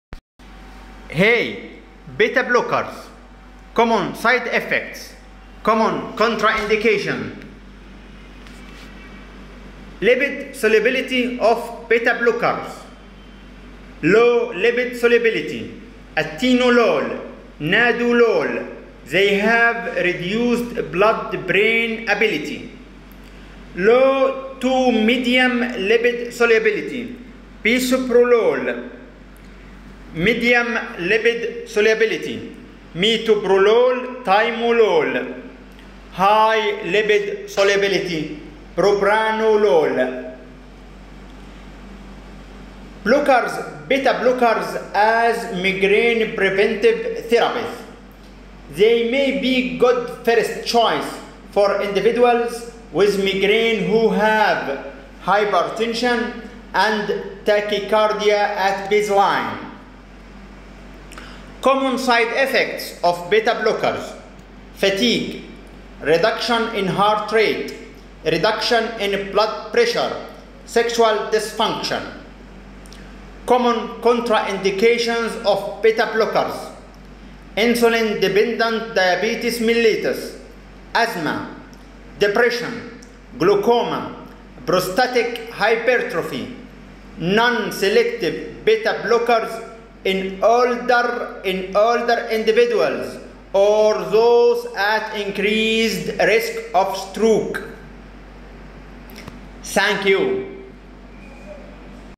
beta blockers and migraine
beta blockers# has many 3side_effects as # fatigue decrease the blood_presure . they has many contraindications as asthma, DM . they are used as a preventive treatment in migriane